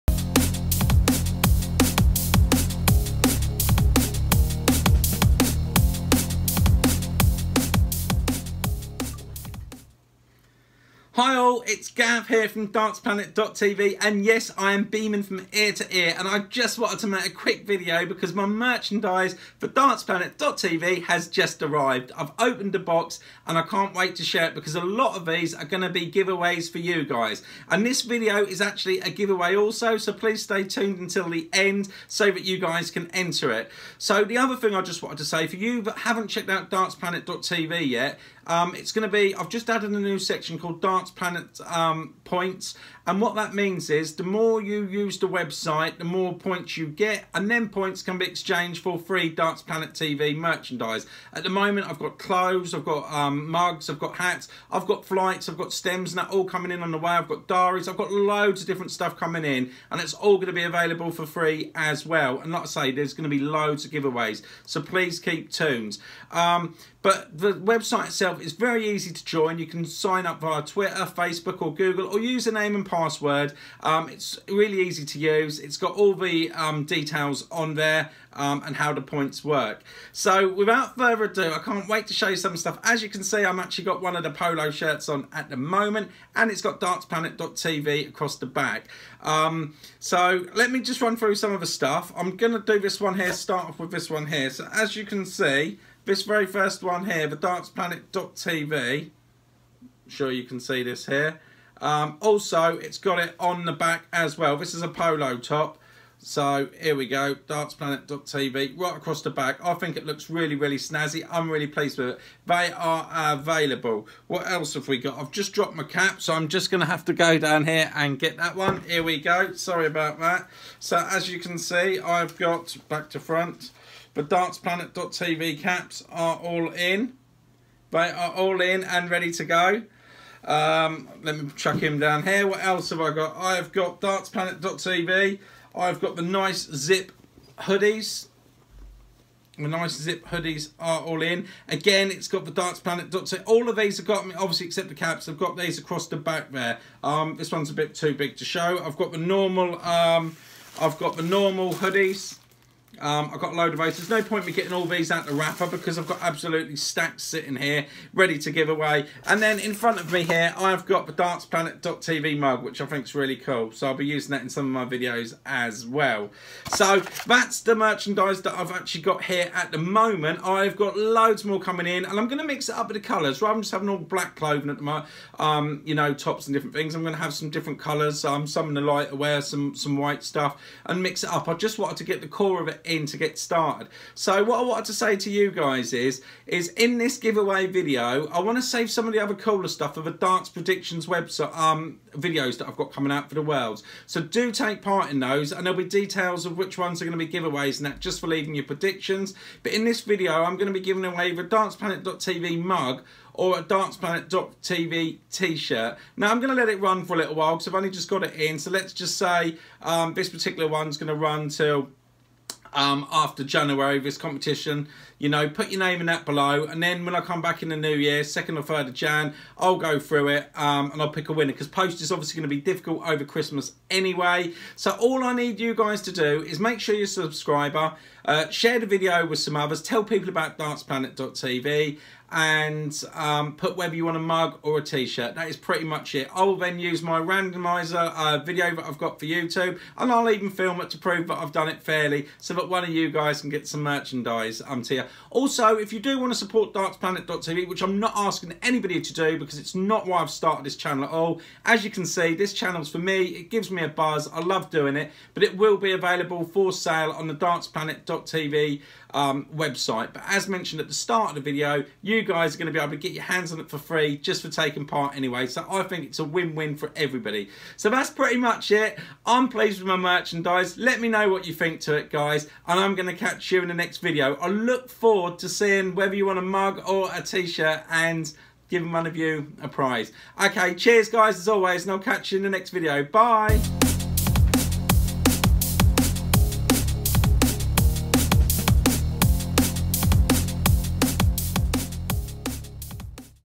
DARTS PLANET TV MERCHANDISE GIVEAWAY - (OPEN) GET YOUR HANDS ON A FREE T-SHIRT OR MUG
My Darts Planet TV merchandise has just been delivered and I can't wait to show you my first lot of goodies as well as do a giveaway.

The giveaway competition ends on the 3rd January and the winner will be picked at random.

Don't forget it will all be available on the Darts Planet TV website and you can also get lots FREE with Darts Planet TV points.

dartsplanet.tv